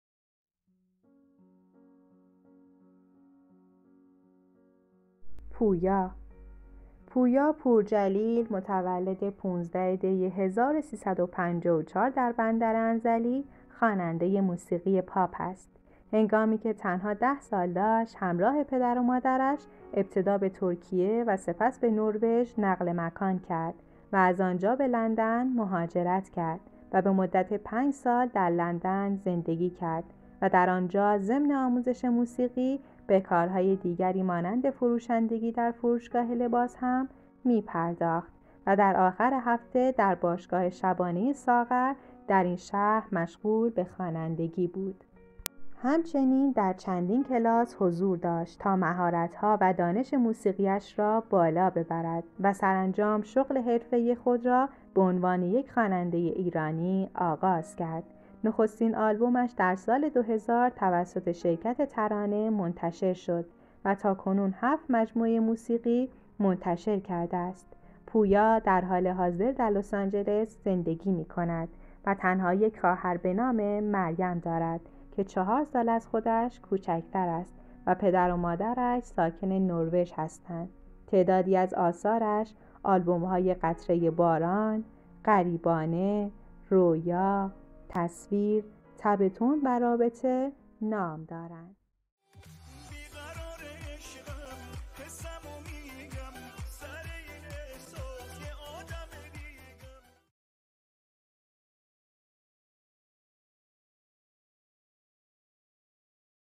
پویا پور جلیل Pouya Pourjalil
💫 معرفی چهره‌های فرهنگ و هنر 💫

پویا پورجلیل (زادهٔ ۱۵ دی ۱۳۵۴ در بندر انزلی) خوانندهٔ موسیقی پاپ است.
Pouya Pourjalil (born January 5, 1976 in Bandar Anzali) is a pop singer.